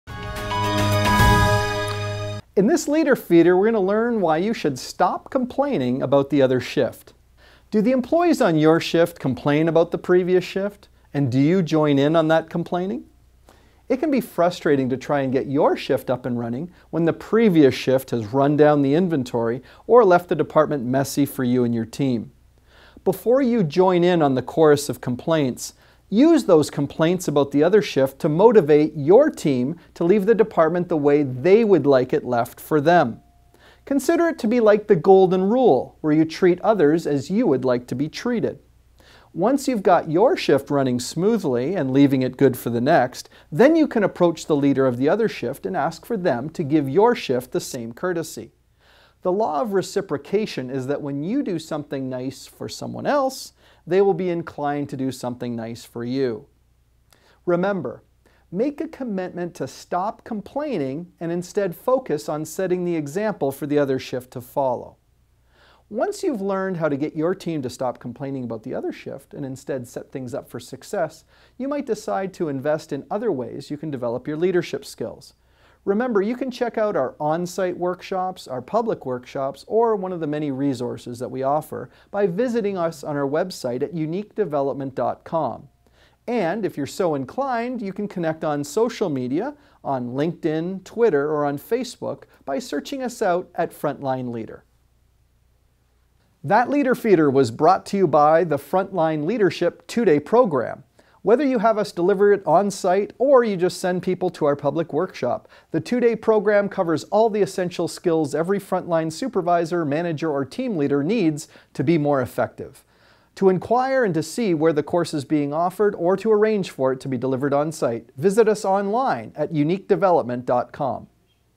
Stop Complaining About the Other Shift
- Does your team complain about how the other shift leaves your department in terms of messiness, low inventory or lack of supplies?

In this leader feeder, we're going to learn why you should stop complaining about the other shift.

When the employees on your shift complain about the previous one, do you join in on that complaining? It can be frustrating to try and get your shift up and running when the previous shift hasn’t left the workplace in ideal conditions.

Before you join in on the chorus of complaints, use them to motivate your team to leave the department the way they would like it left for them. Consider it to be like the “golden rule” where you treat others as you would like to be treated.

Once you've got your shift running smoothly and are consistently leaving it in good shape for the next shift, you can approach the leader of the other shift and ask that they give your shift the same courtesy.

The law of reciprocation is that when you do something nice for someone else, they will be inclined to do something nice for you.

Complaining will often lead to more of the same.  So remember to make a commitment to stop complaining and instead focus on setting the example for your team as well as the other shifts.

A proactive and positive solution creates a win-win scenario for everyone.

What’s one way you create harmony across different shifts in your workplace?